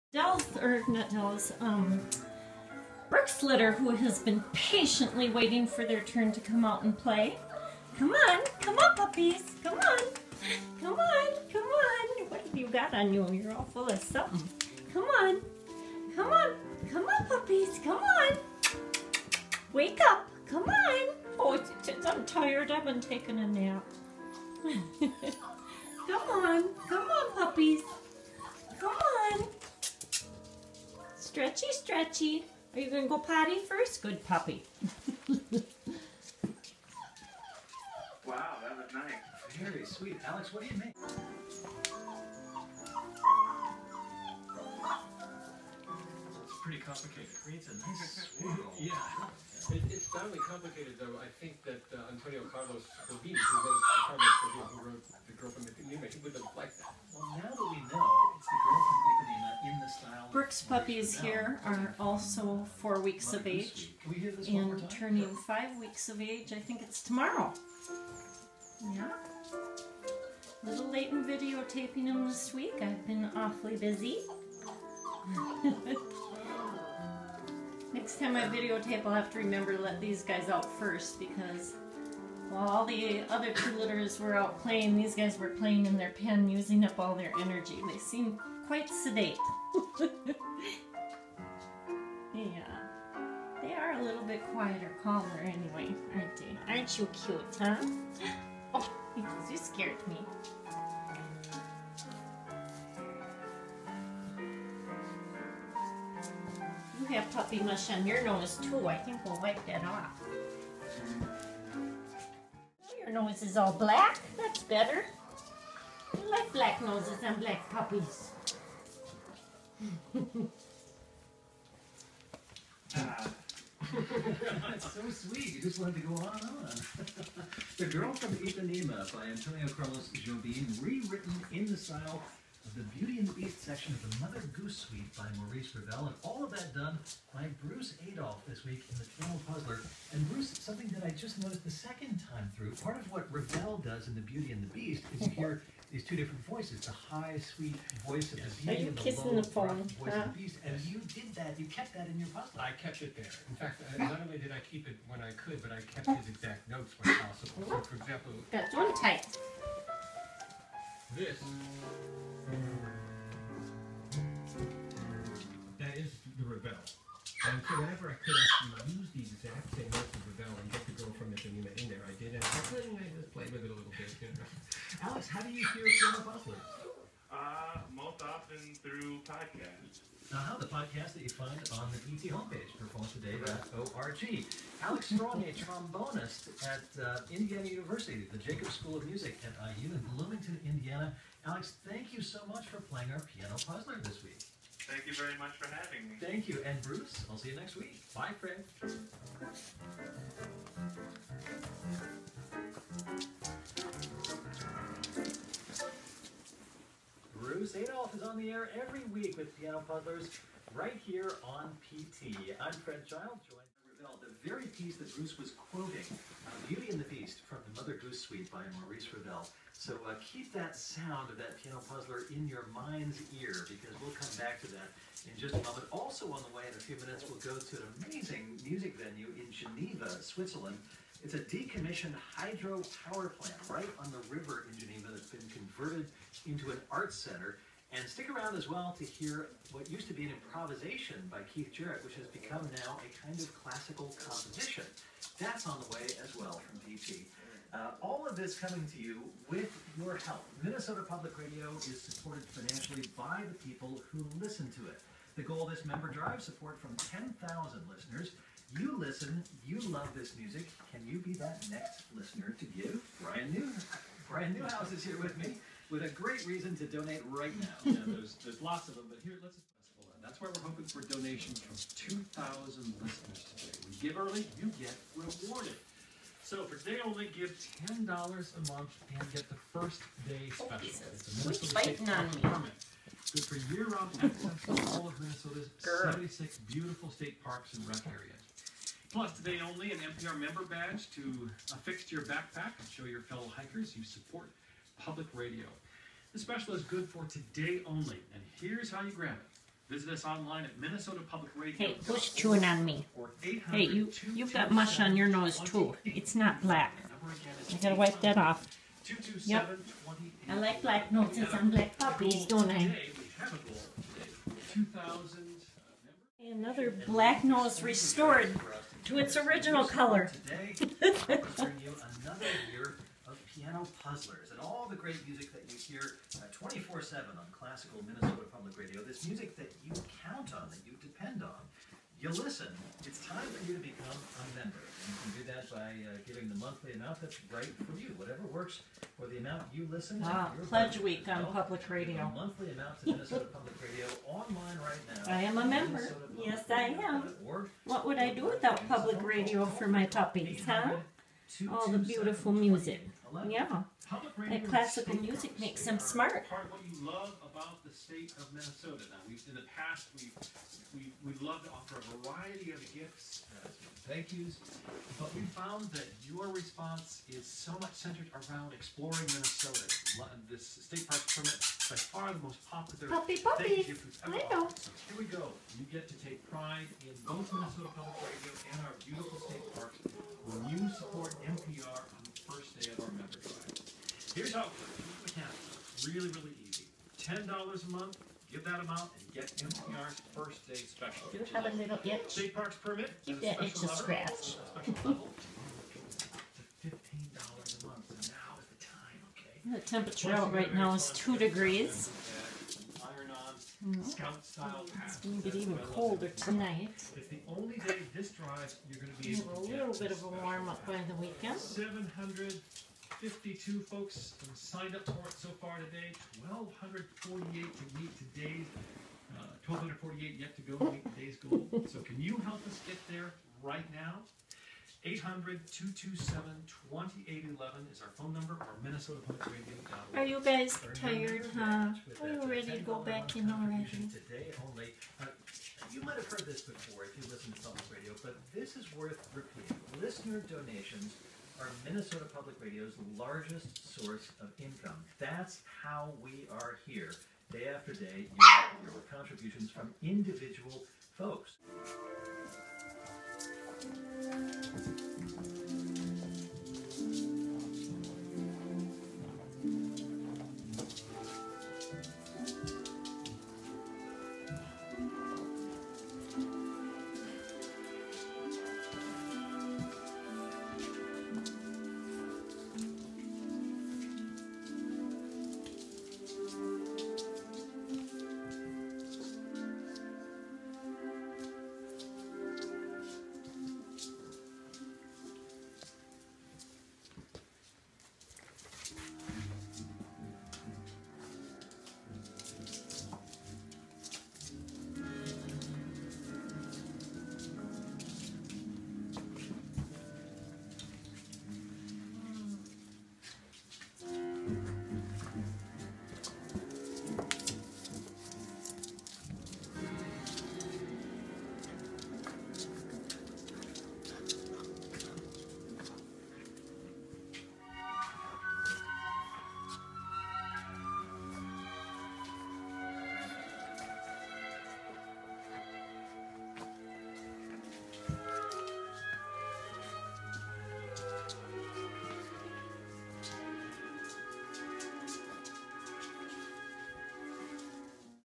Brook x Del 4-week-old British Labrador Puppies
Haggisbank Brook of Queensland (a.k.a. Brook)
x Minstead Delvin (a.k.a. Del)
Whelp Date: Thursday January 2, 2014
Status: 7 Puppies: 6 Black Males and 1 Black Female